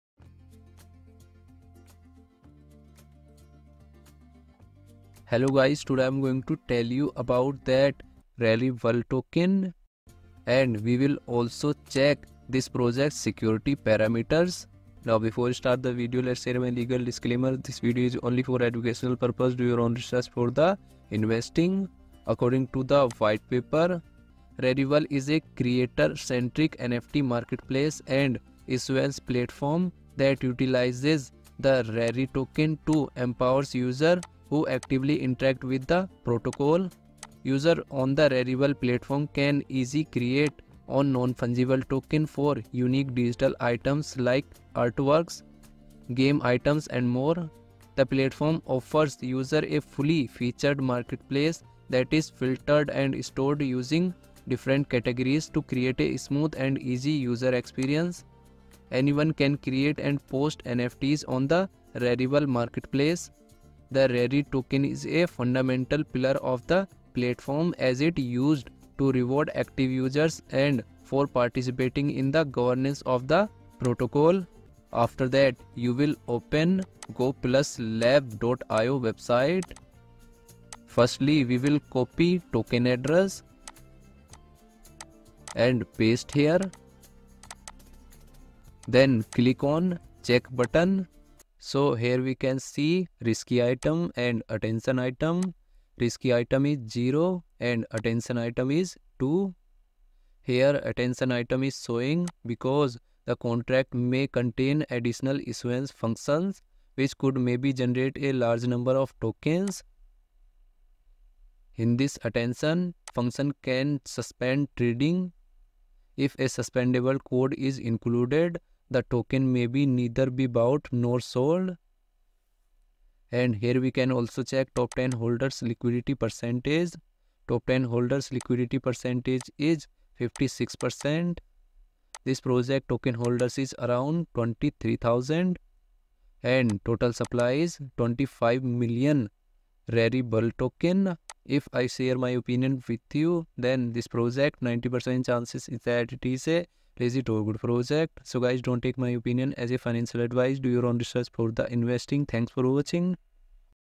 Is Rarible (RARI) token Good Or Not | Review About RARI Token
In this video, I will share a Review of the Rarible (RARI) token and security parameters for the project good or not. I have collected all the data from online sources please don't take this video as financial advice. I am not a financial advisor.